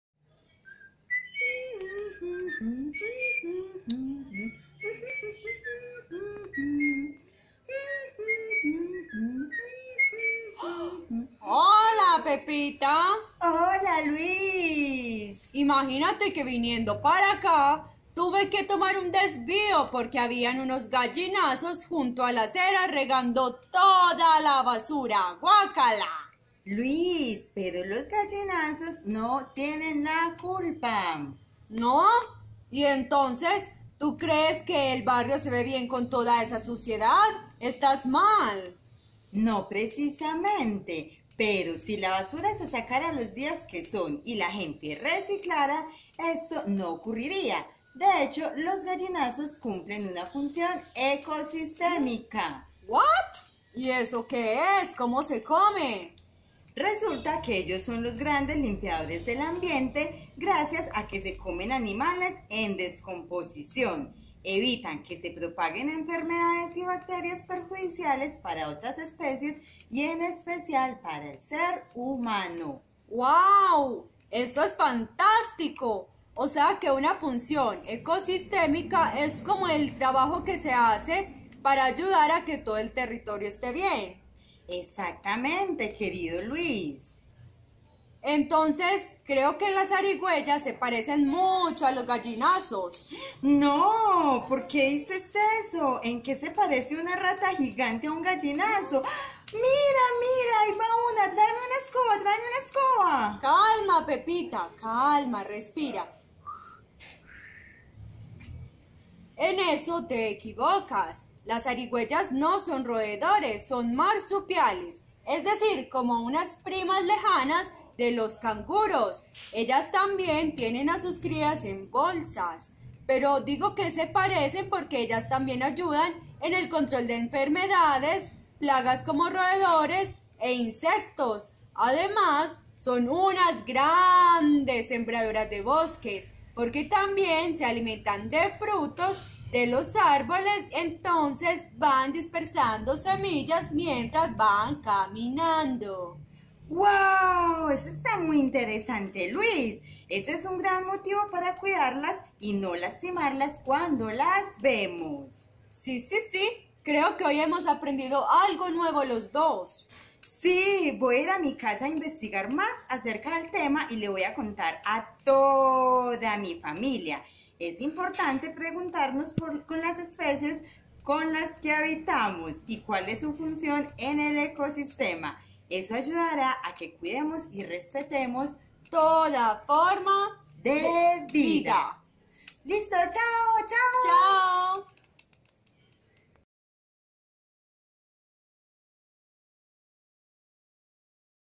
Personaje de la semana: Zarigüeya y Gallinazo
En ocasiones juzgamos otros seres y tenemos prejuicios por su aspecto o por los lugares en donde los vemos, desconociendo sus funciones ecológicas, hoy te presentaremos la importante función del gallinazo y la zarigüeya.